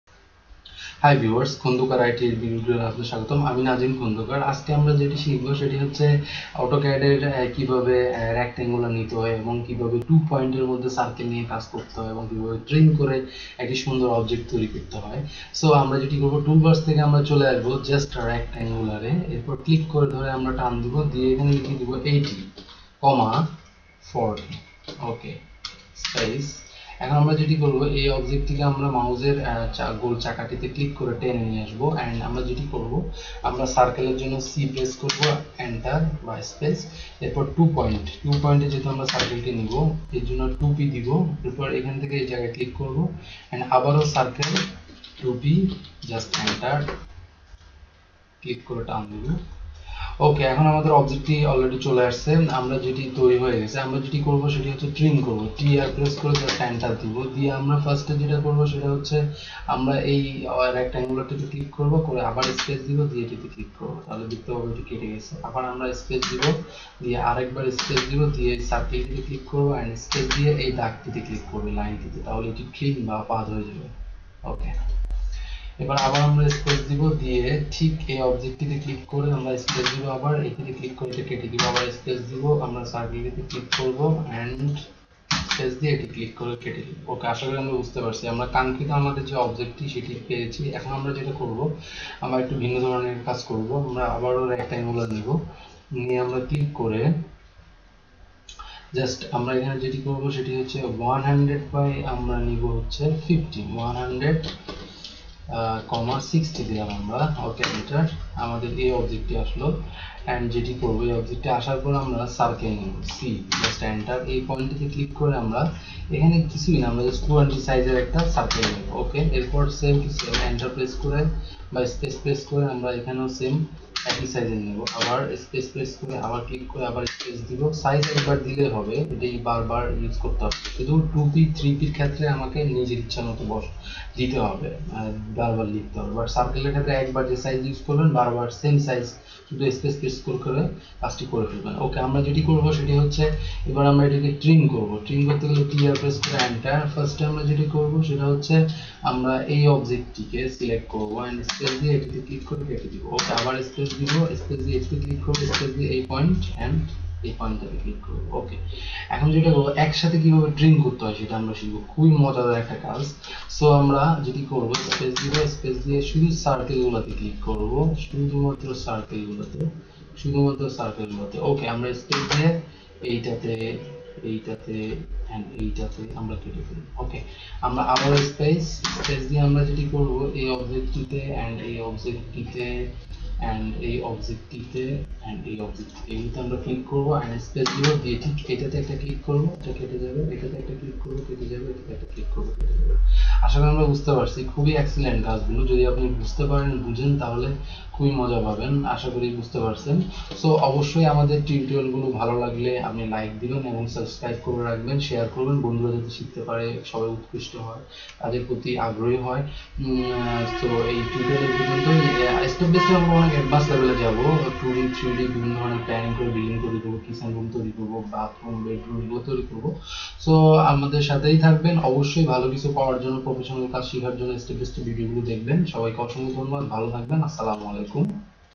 AutoCAD 2D procedure of Rectangular, Circle 2p & Trim. bangla tutorial Class 6
এই ভিডিওতে AutoCAD 2D এর Rectangular, Circle 2p & Trim কিভাবে ব্যবহার করে কার্য সম্পন্য করবেন তার সম্পর্কে আলোচনা করা হয়েছে ।  মনেযোগ দিয়ে শিখুন । আশাকরি ভালো করে শিখা সম্ভব হবে । ধন্যবাদ । নিজে শিখুন এবং সহপাঠিকে শিখতে উৎসাহিত করুন ।